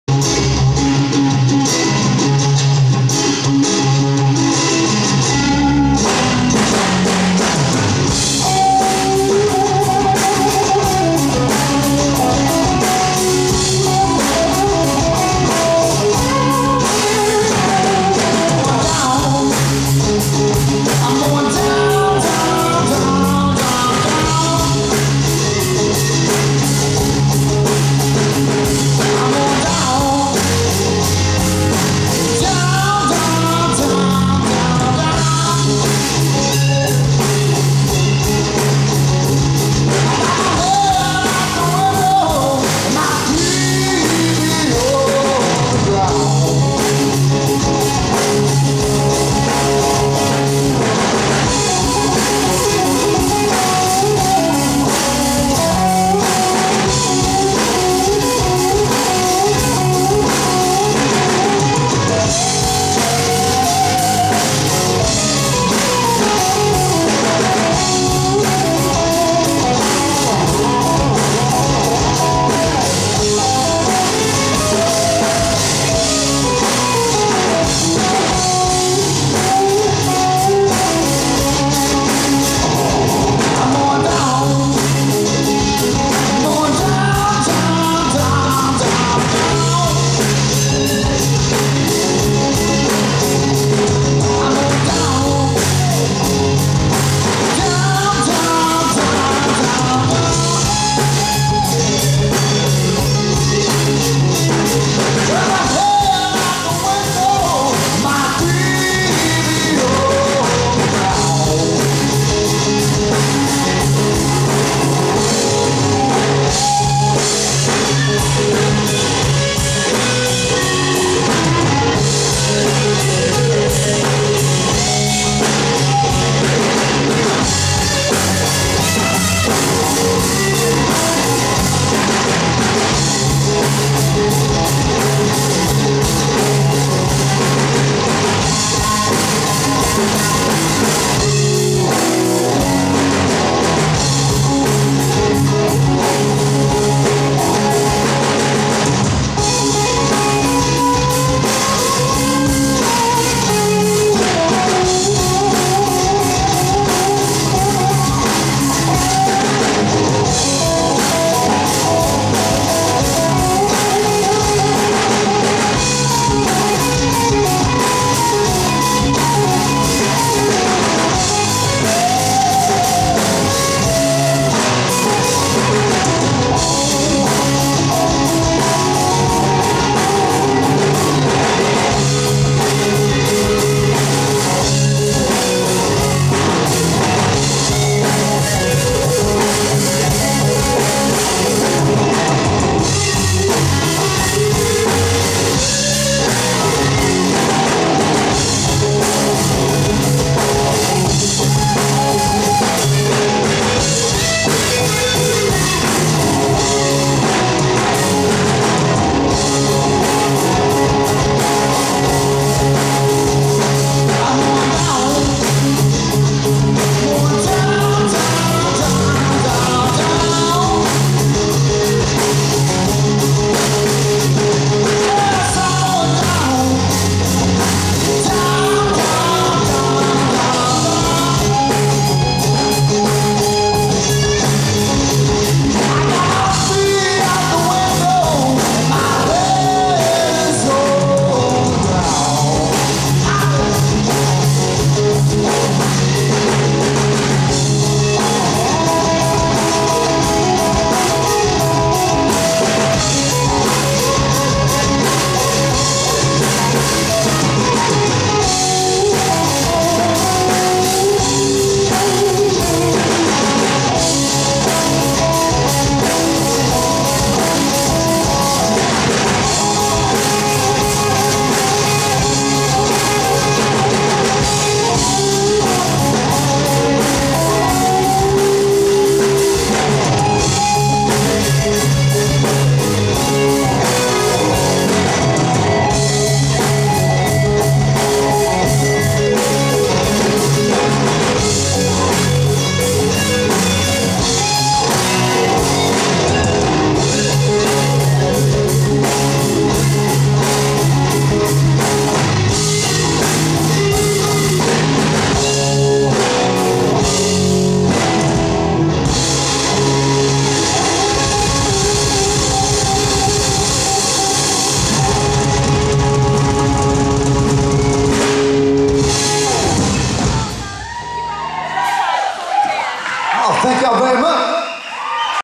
Blackwater live @ Brewsky's: 8
Filmed by: Downtown Music Media

Jacob Arnold: Lead vocals & guitar
Paul Hagelston: Bass
Tony James: Drums